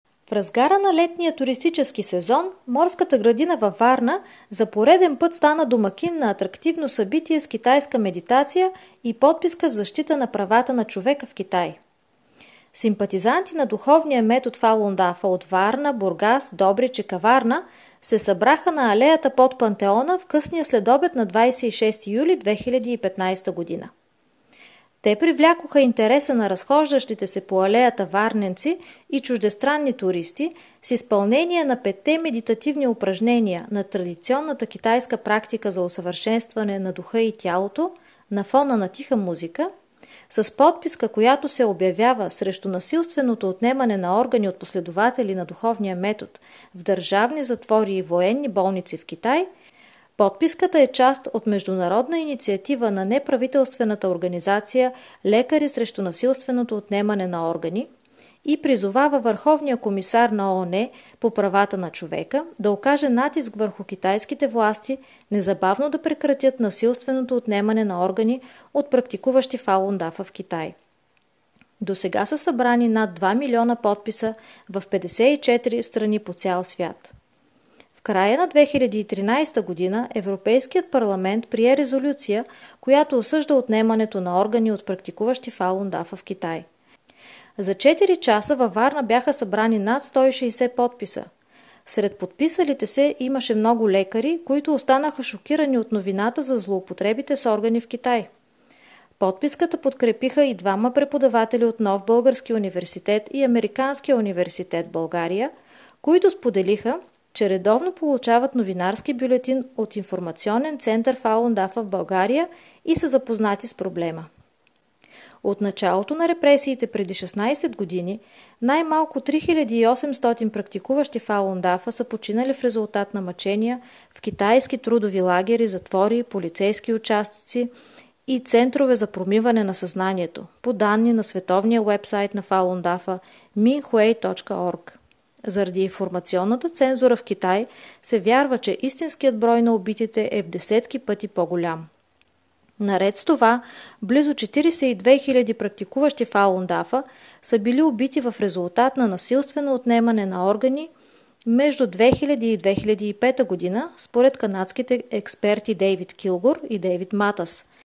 Представяне на Фалун Дафа във Варна
Симпатизанти на духовния метод за подобряване на духа и тялото Фалун Дафа от Варна, Бургас, Добрич и Каварна се събраха на алеята под Пантеона в Морската градина във Варна на 26 юли, 2015 г. и привлякоха интереса на разхождащите се варненци и чуждестранни туристи с изпълнение на петте медитативни упражнения на практиката на фона на тиха музика, подписка в защита на човешките права в Китай и мирно бдение със свещи.